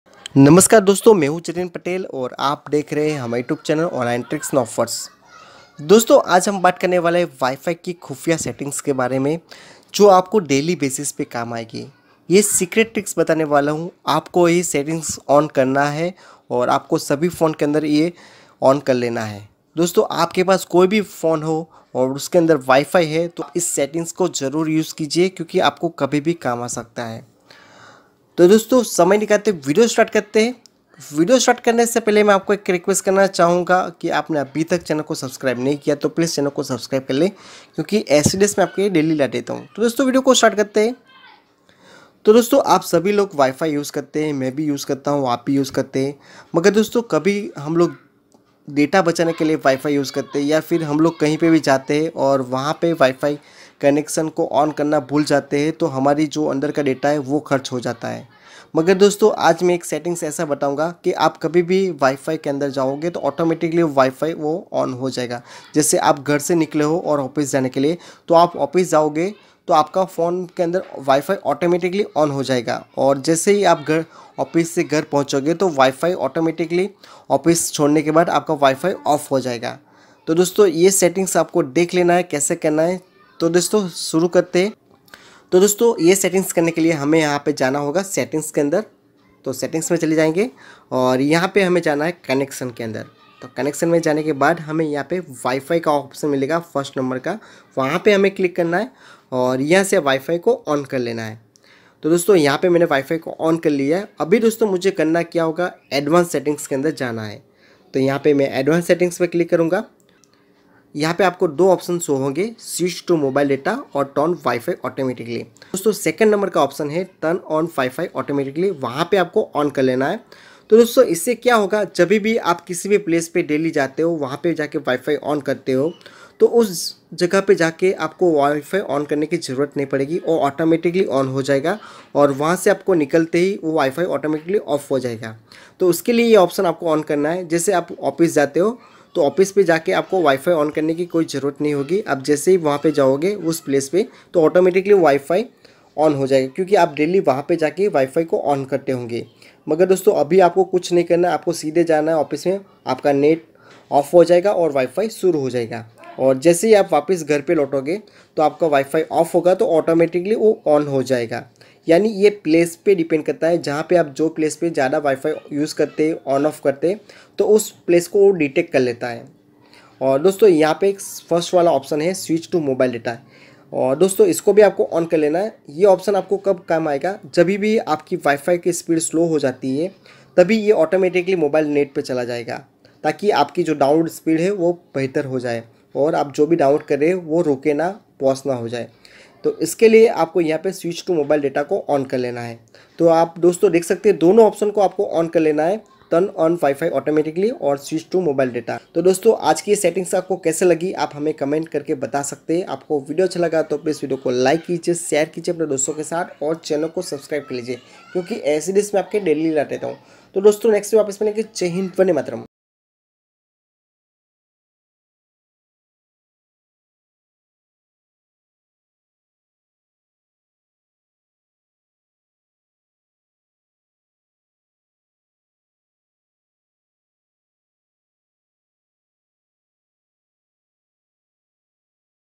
घर पे जाये या ऑफिस जाये WiFI अपने आप On हो जायेगा Mobile में और Off भी हो जायेगा. WiFi Tricks 2020.
ABOUT

1- Filmora (on pc)
2- kineMaster (on mobile)

Video Thumbnail i Use

1- PixeLab
2- PicsArt
3- Snapseed